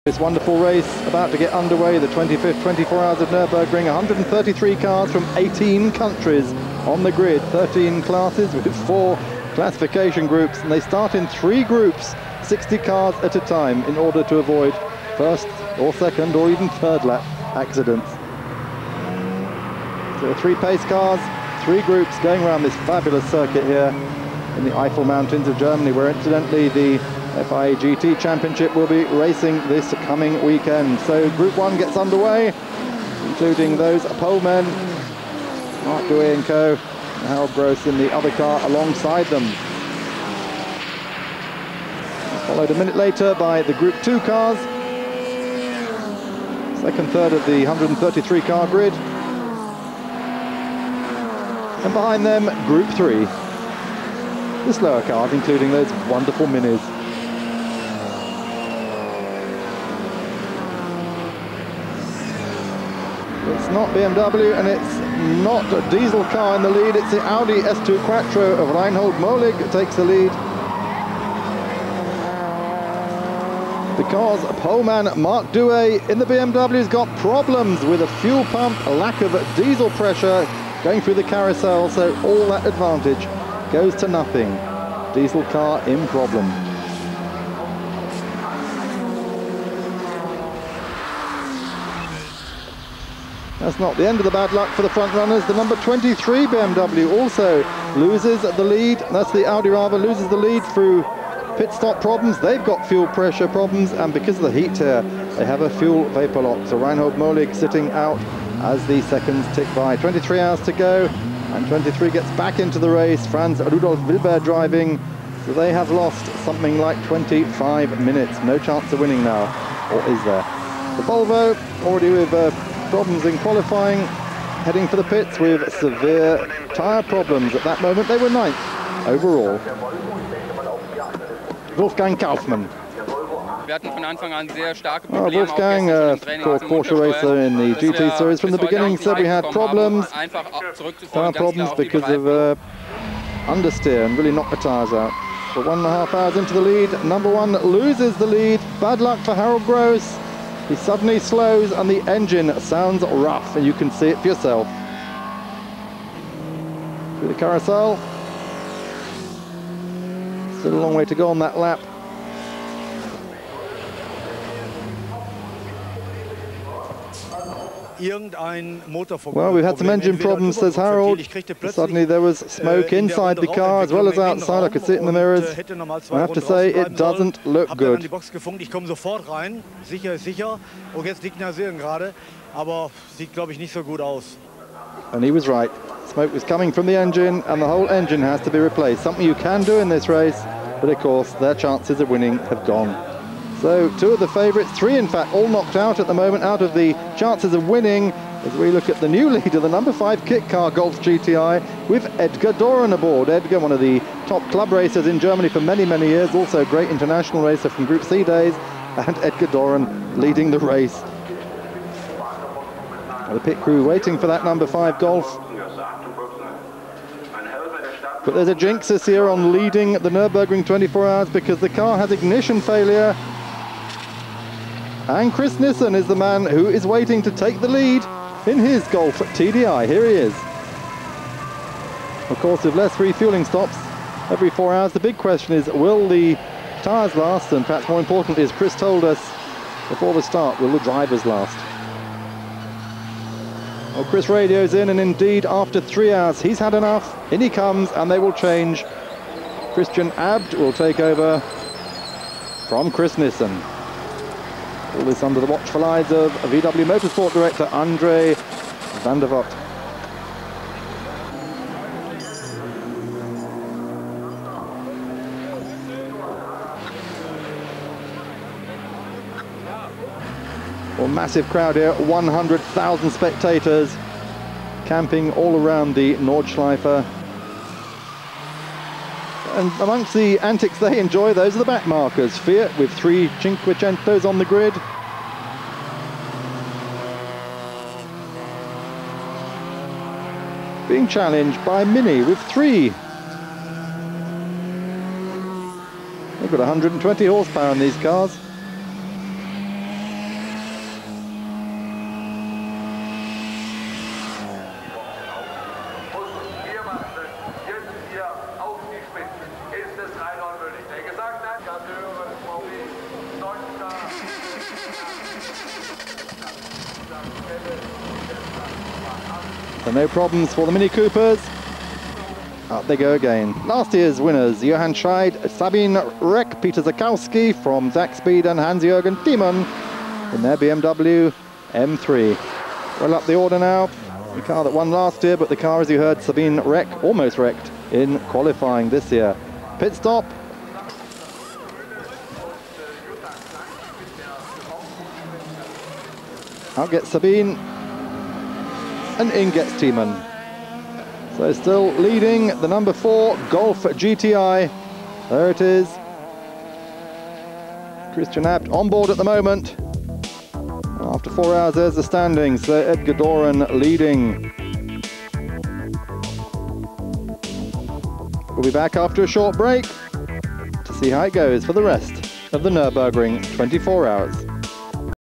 1997 Nurburgring 24 hours part 1.
Part 1 on the race coverage of the 1997 Nurburgring 24 hours.